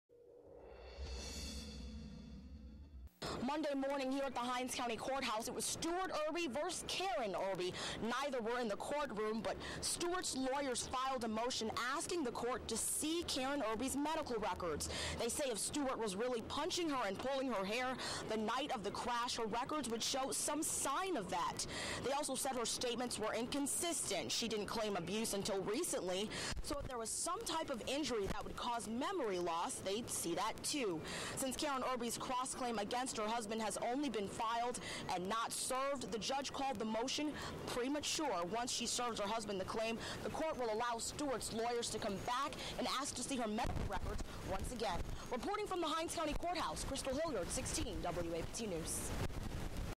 Stuart Irby Asks For Wife's Medical Records
Stuart Irby's attorneys ask to see his wife's medical records.